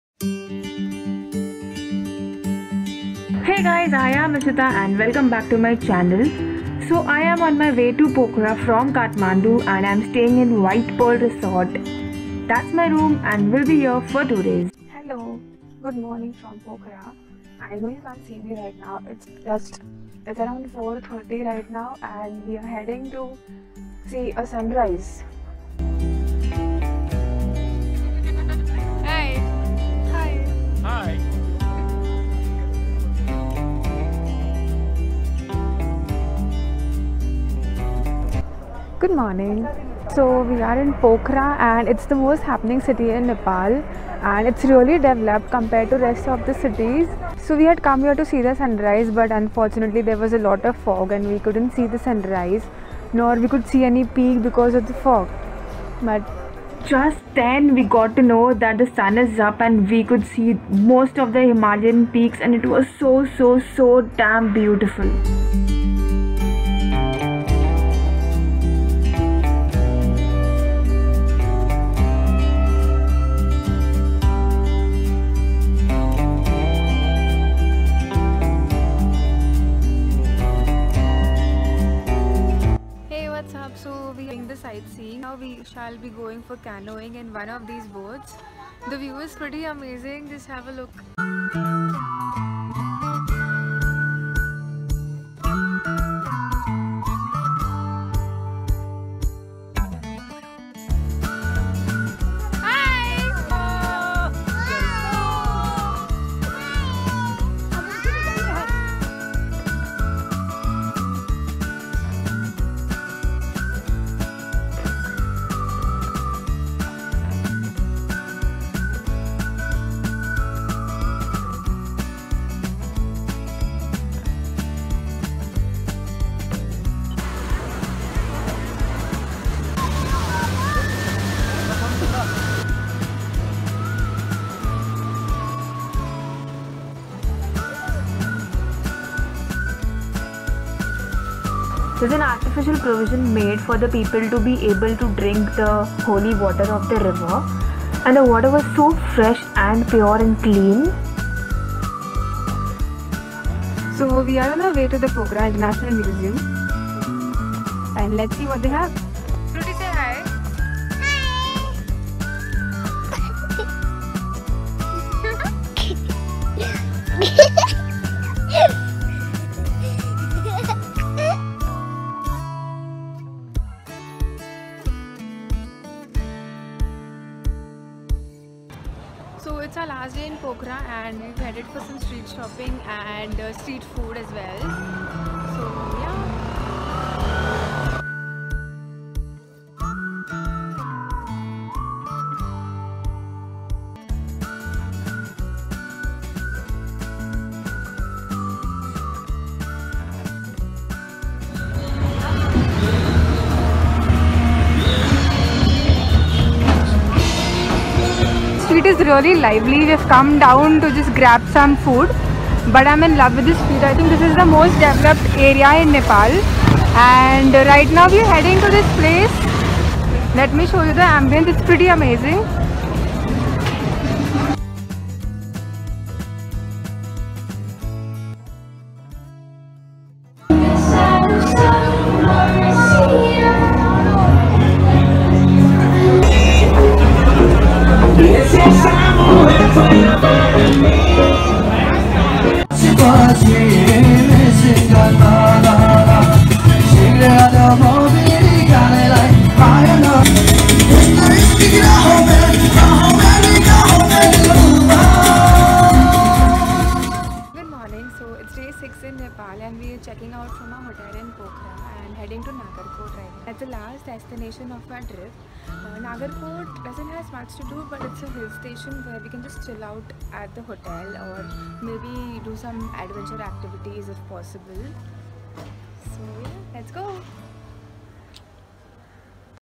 Nepal - POKHARA | Nightlife , Himalaya, Sightseeing
POKHARA  travel Vlog from My Nepal Travel Vlog series.
Most scenic Canoeing experience , Sunrays reflecting on the Himalayan peaks , Night life in Pokhara along with the sightseeing !!!

Last EPISODE COMING SOON!!
Hit Like if you enjoyed watching this Video.

ISHITA AGRAWAL
Connect :